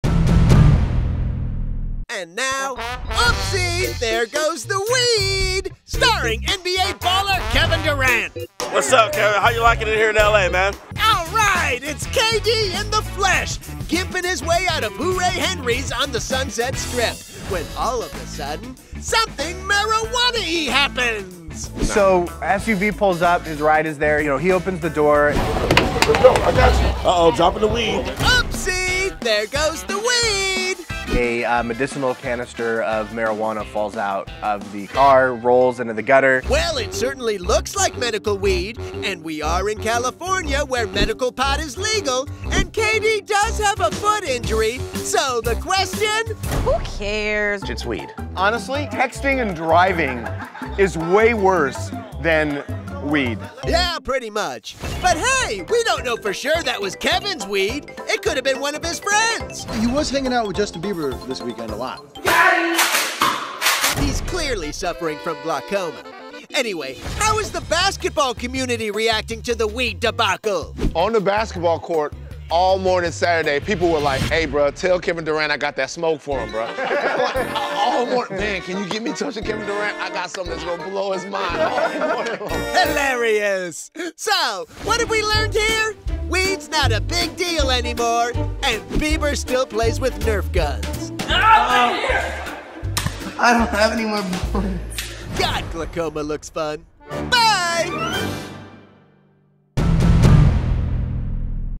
Kevin Durant- Oopsie! There Goes The Weed! | TMZ
NBA Star KD opened the door to his SUV and some medical weed fell out!  Is it his?! Click 'SHOW MORE' for related content...